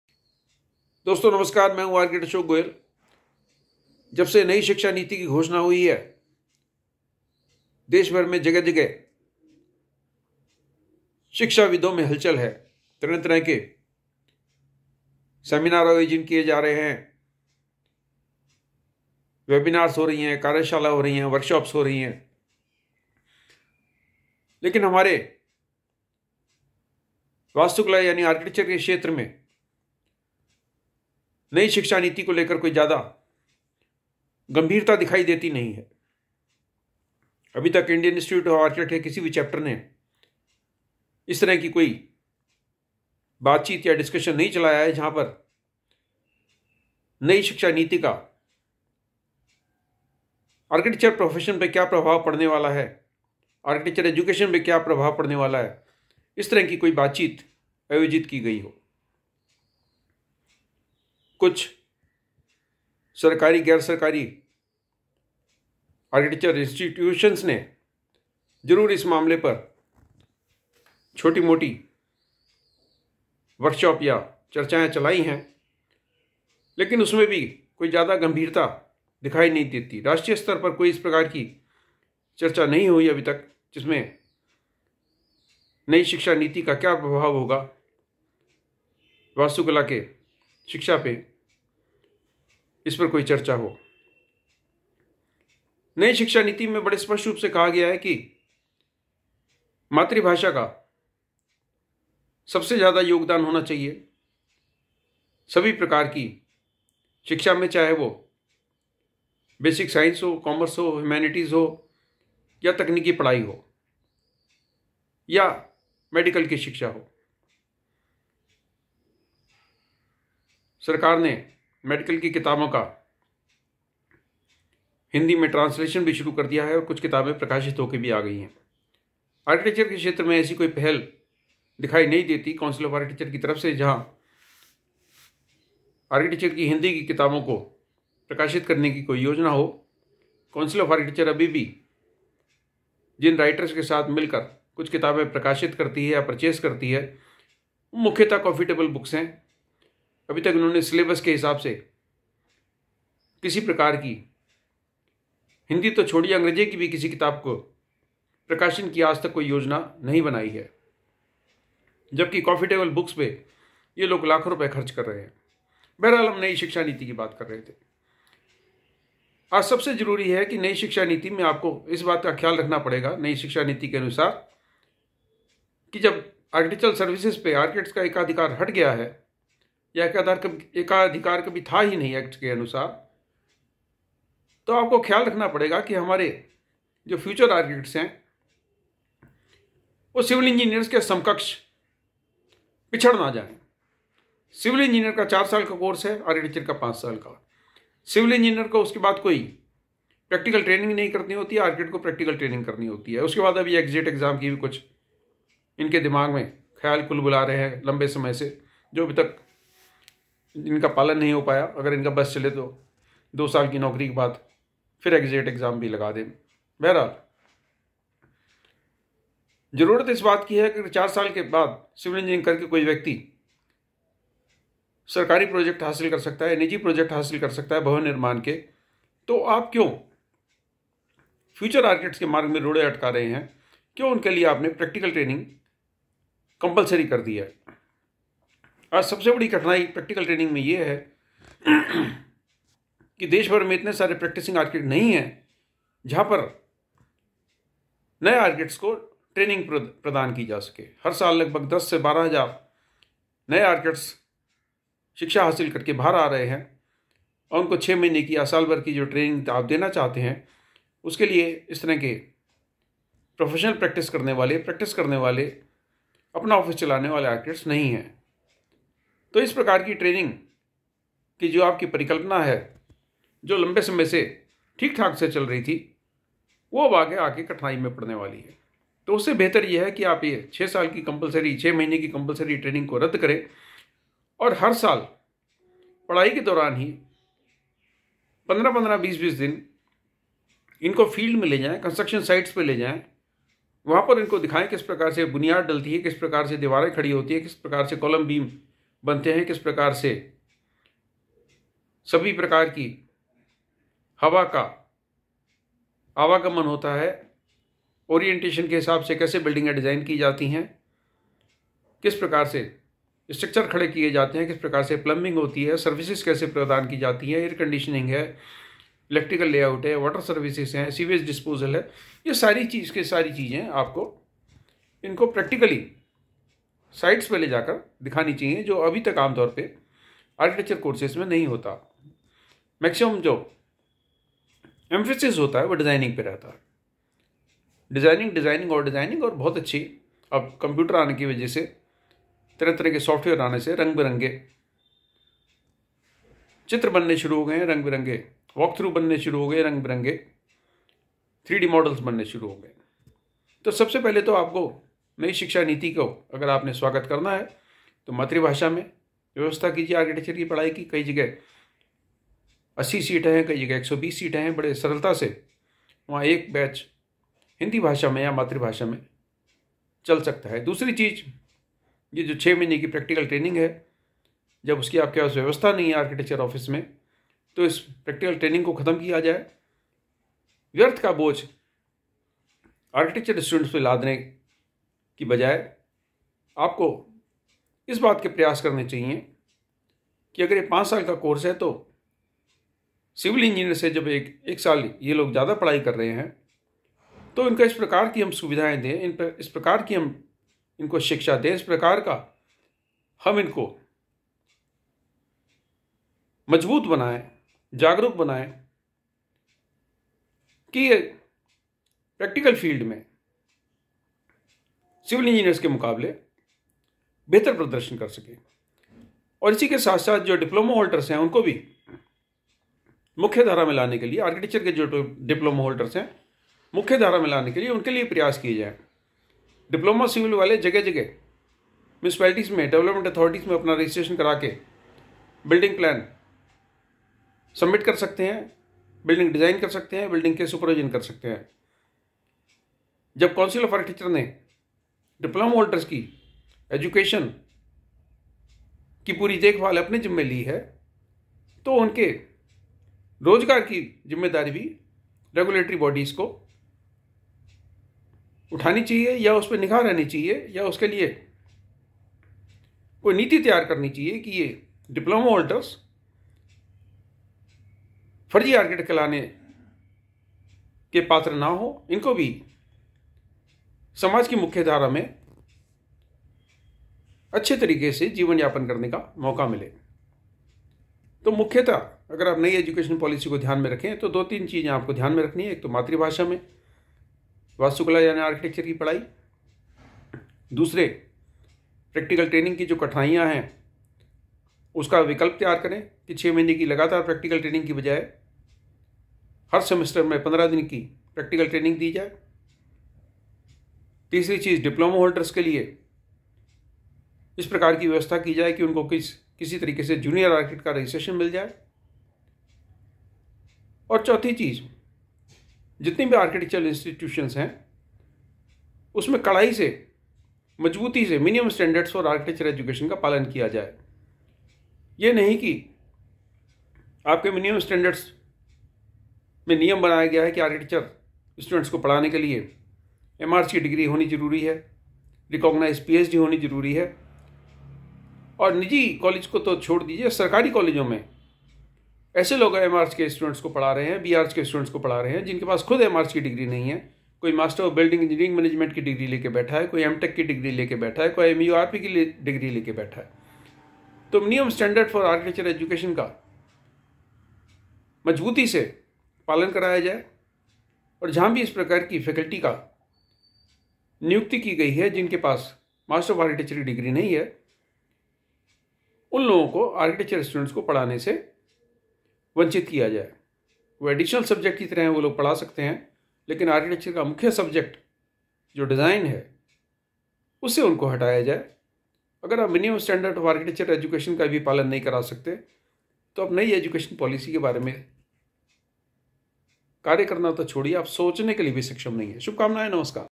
Architecture and New Education Policy.
Architecture Education should be in harmony with New Education Policy.
Education in mother tongue and education for all should be the most important factors to be followed for architecture education.